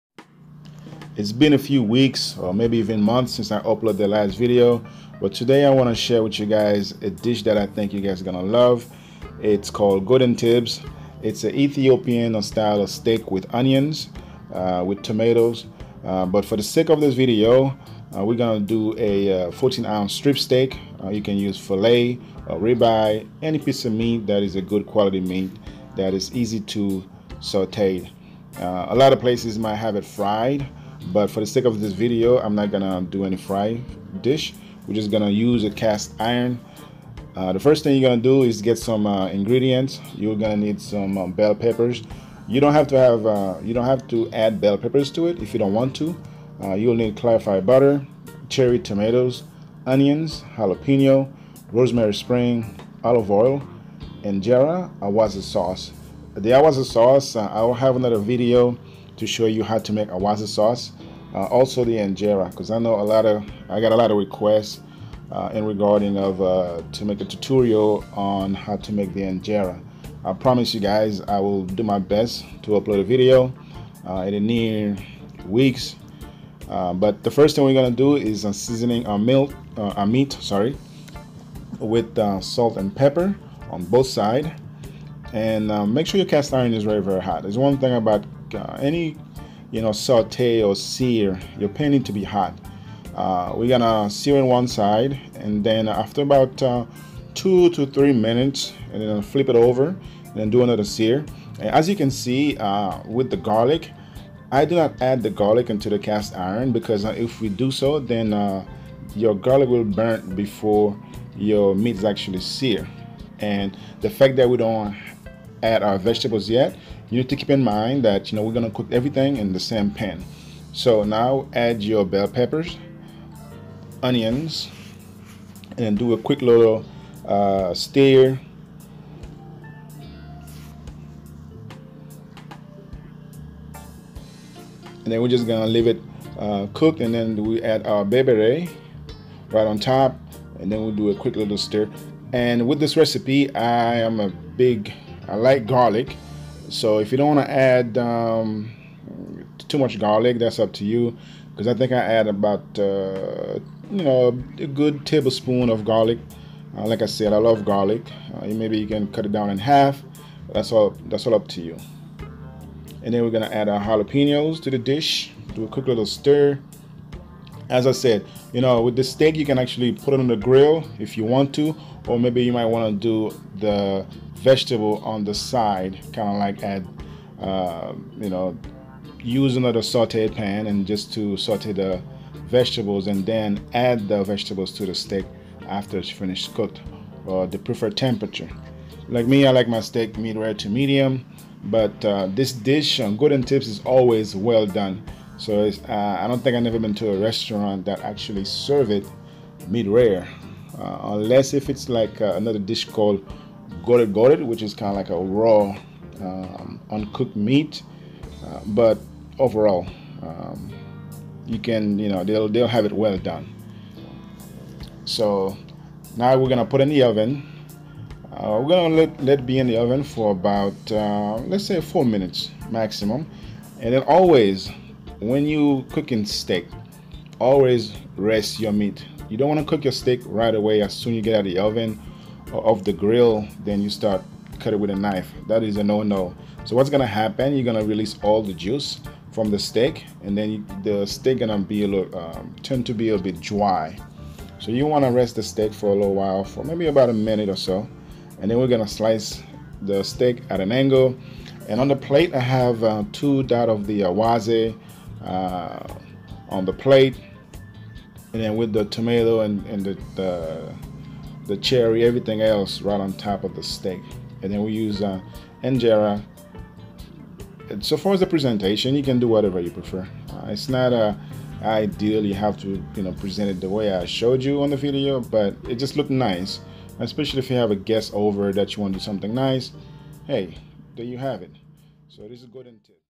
How to make Ethiopian Steak-Goden Tibs
Ingredients
14 oz strip steak
6 slices bell peppers
1 tbsp chopped garlic
1 oz  Onions slice
1tsp berbere spice
5 cherry tomatoes
4 jalapenos slice
1 tsp olive oi
1 tbsp spiced clarified butter
1 rosemary spring
Pinch Salt & Peper to taste
1 tsp Berbere
1 tbsp Awaze sauce
1 Injera (Ethiopian flatbread)

Seasoning meat on both side with salt & pepper, heat up the pan to high-temperature place meat in the pan for about 2 minutes.  Flip over adds half of the garlic on top, then add onion, jalapenos, rosemary,  on one side of the pan add clarified butter, berbere and a little of salt to taste. Flip steak over adding the rest of the garlic finished in the oven for about 3 or 4 minutes. Let the meat rest for about 2 before cutting. Place sliced meat to a plate and awaze sauce, next to injera.